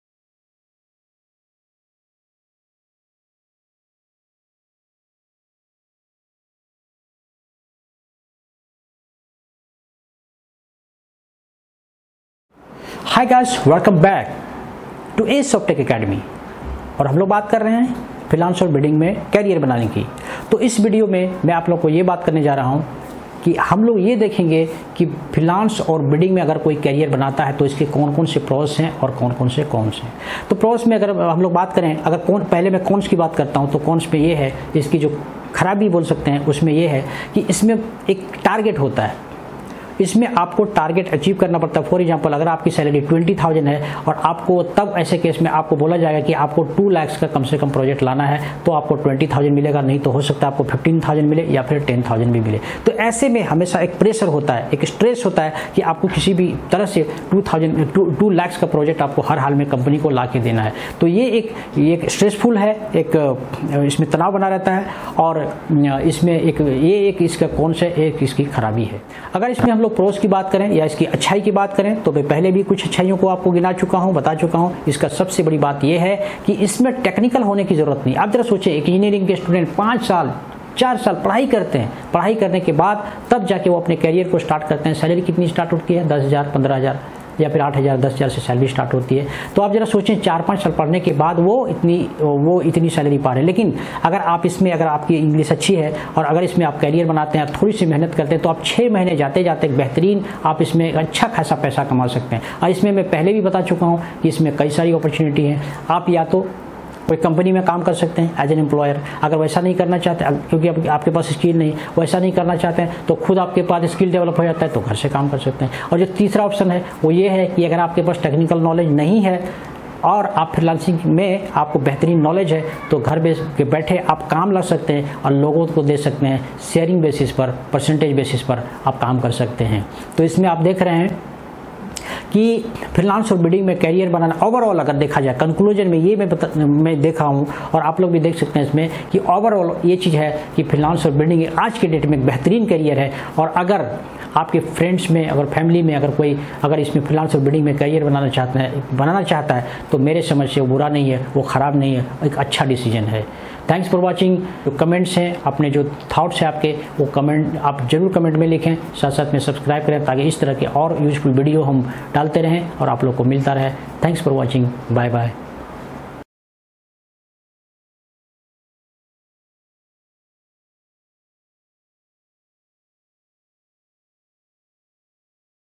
Pros and Cons of Freelance and Biding career | Freelancer Training - Part-12
There are two sides to every coin. Even in this career also there are pros and cons.
Through this video, i will mention some of the pros and cons.

There many advantages or pros in this career.
In this field, you don't need to have the technical knowledge, basic knowledge is enough so that you can communicate to the clients.
You have many options to work. You can even work for a company, you can work as an freelancer and  do bidding for yourself. You can even work as a project bidder and can outsource the work to a freelancer.

First advantage or con of freelancing and bidding is that it is target oriented work. You have to achieve that target to get salary. There is pressure when it comes to work because the mork work you bring the more you can earn